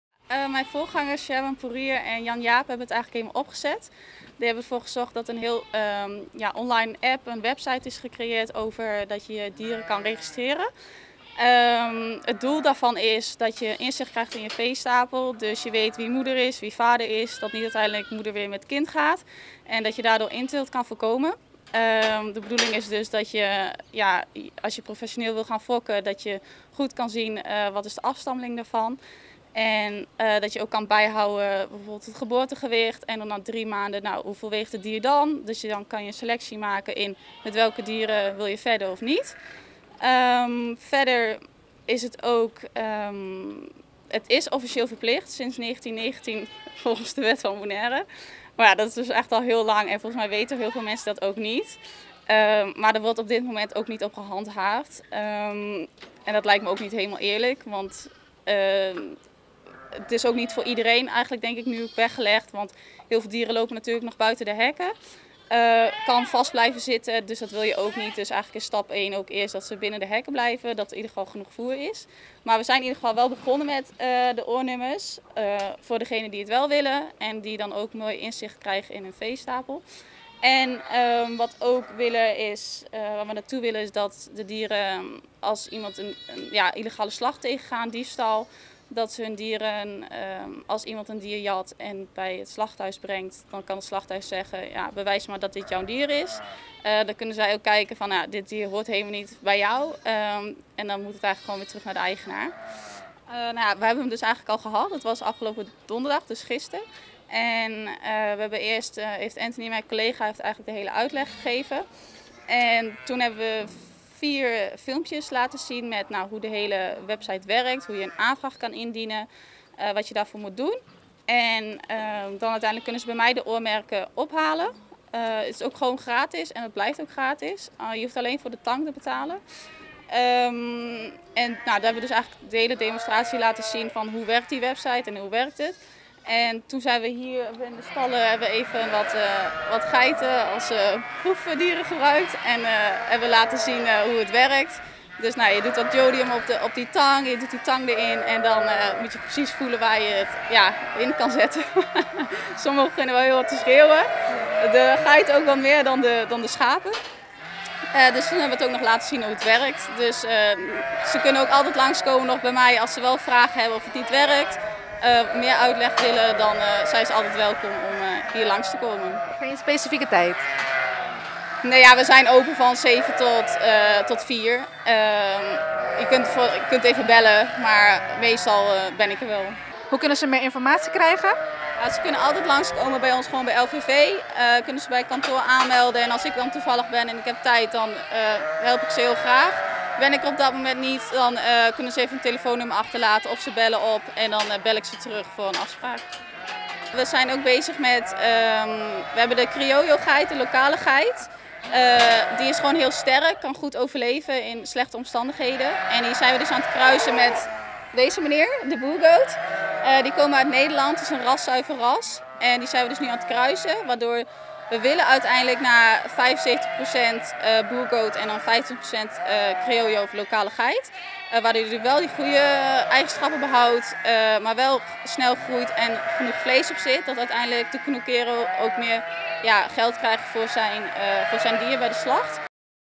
Registrashon online di kabritu, karné i markamentu di e bestianan
LVV a organisá dos enkuentro pa informá doñonan di kabritu i karné tokante e sistema di identifikashon i registrashon. E sistema ta un kombinashon di registrashon online di kabritu i karné i markamentu di e bestianan. Hanna Bergsma, lider di proyekto krio di bestia na LVV ta duna mas detaye.